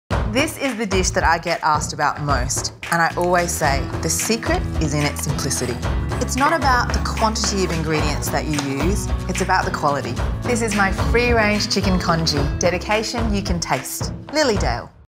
Dedication You Can Taste with Poh Ling Yeow - Chinese Chicken Congee
Try Poh Ling Yeow's simple and delicious Chinese Chicken Congee with Lilydale's free-range chicken.